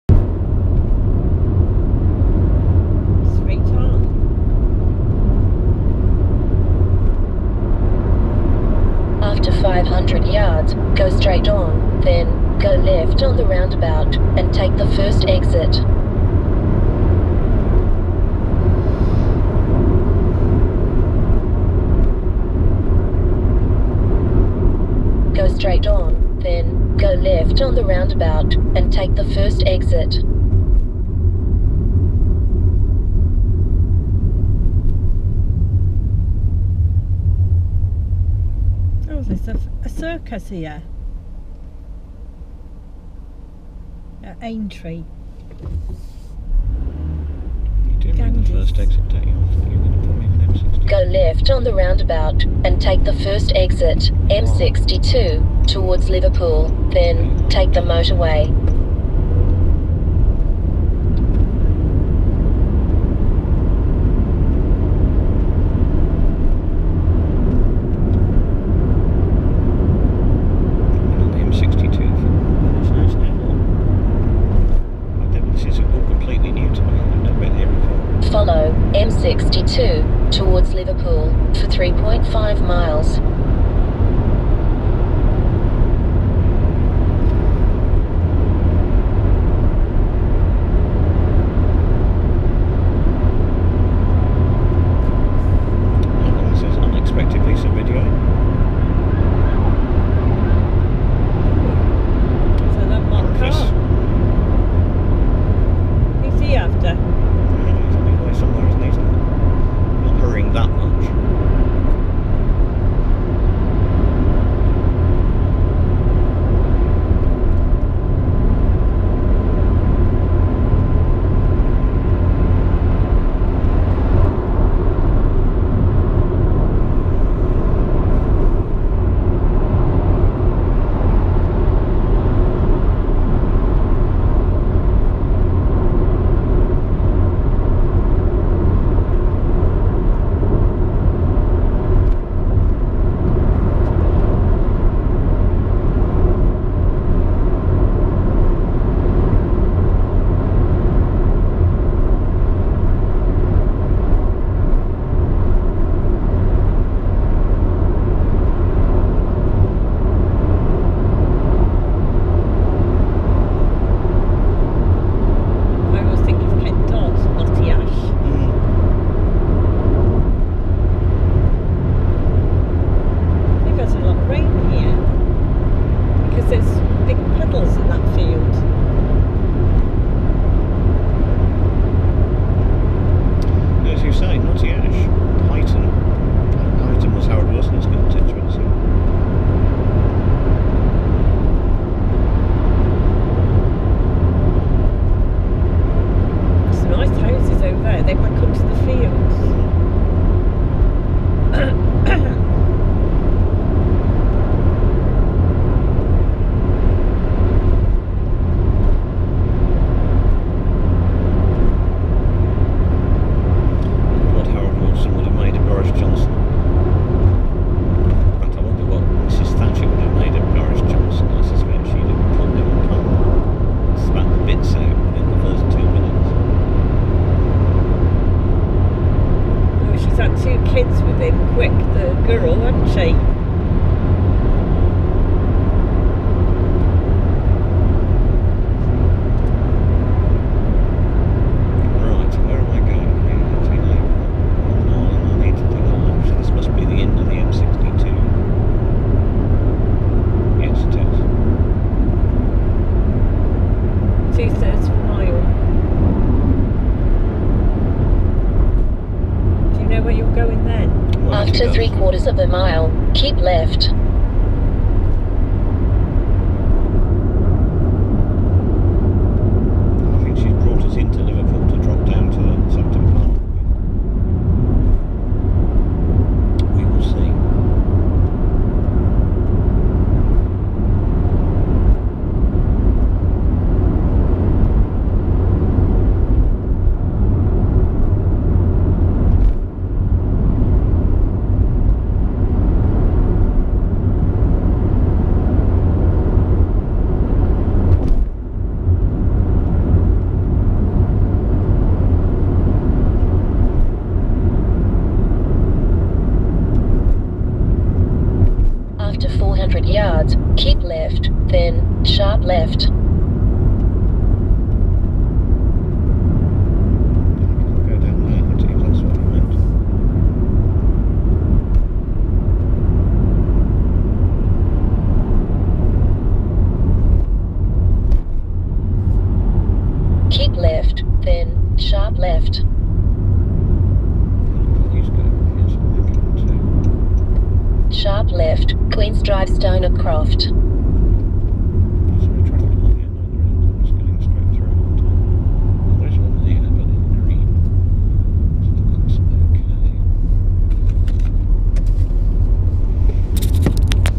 UK Motorways - M62 Liverpool - J7 to J4
In this video, part of the UK Motorways series, we are on the M62 in Merseyside from J7 (Mersey Gateway) to J4